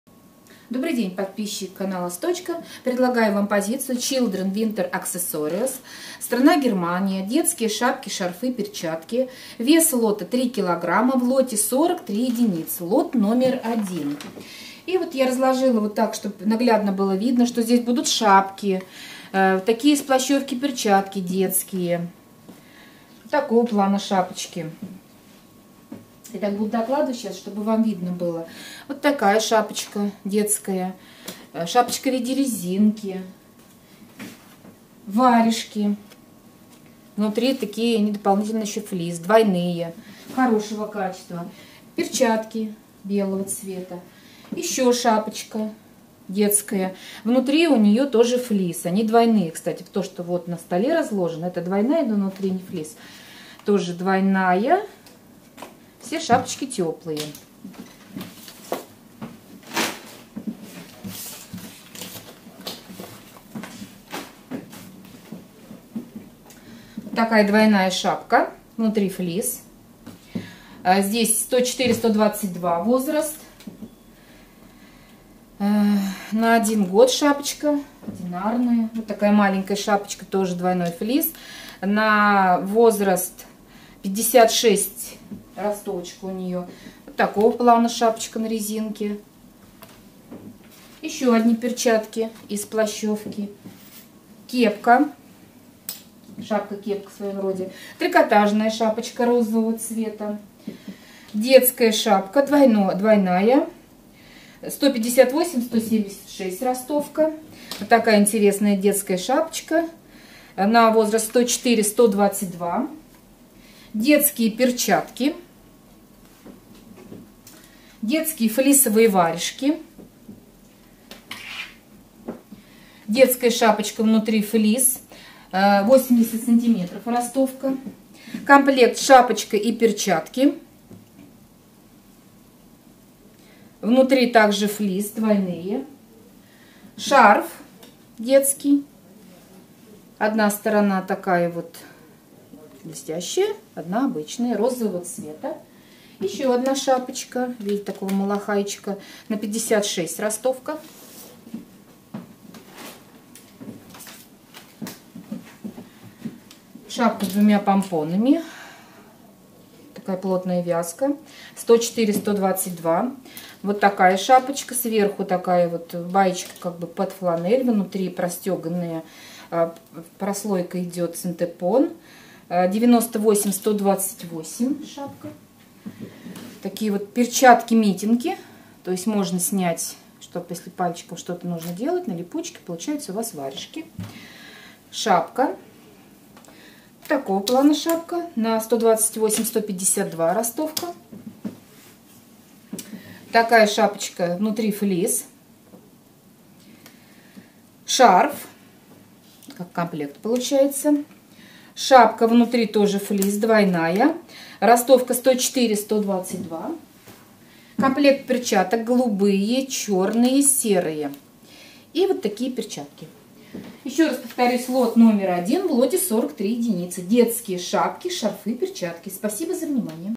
С&A СHILDRENS WINTER ACC 1, Цена 13.42 евро за кг, сток одежда оптом
С&A СHILDRENS WINTER ACCESSORES (лот №1)
Зимние детские аксессуары (сток)
Страна: Германия
Сезон: Зима
Цена за кг.: 13.42 евро
Цена за мешок: 40.26 евро
Цена за ед.: 0.94   евро